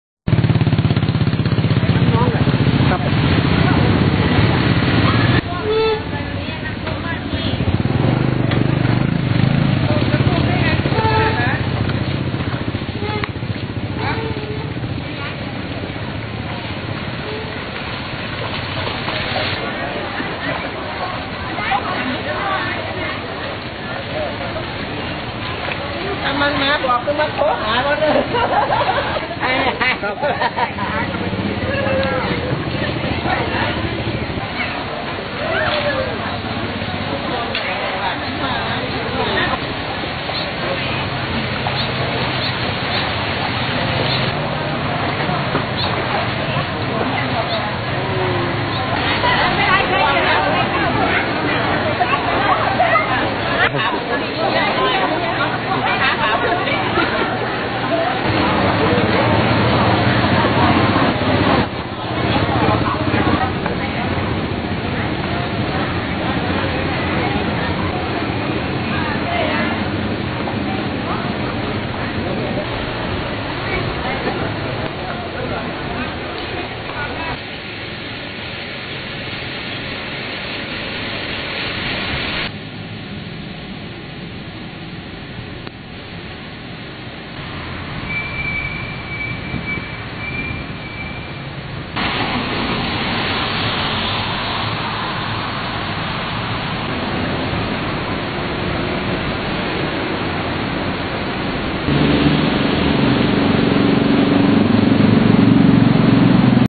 น้องน้ำมาแล้ว..ดอกคำใต้จมบาดาล พ่อค้าแม่ค้า-ชาวบ้านต้องลุยน้ำจ่ายตลาด
1807/2017 น้องน้ำมาแล้ว..ดอกคำใต้จมบาดาล พ่อค้าแม่ค้า-ชาวบ้านต้องลุยน้ำจ่ายตลาด
พะเยา - น้ำร่องช้างล้นตลิ่งตั้งแต่กลางดึก ก่อนทะลักท่วมพื้นที่เขตเทศบาลฯ ดอกคำใต้ จนพ่อค้าแม่ขาย-ชาวบ้านต้องลุยน้ำจ่ายตลาดกันตั้งแต่เช้าตรู่ จนท.ประกาศเตือนขนของขึ้นที่สูง-ปิดถนนข้างโรงพักแล้ว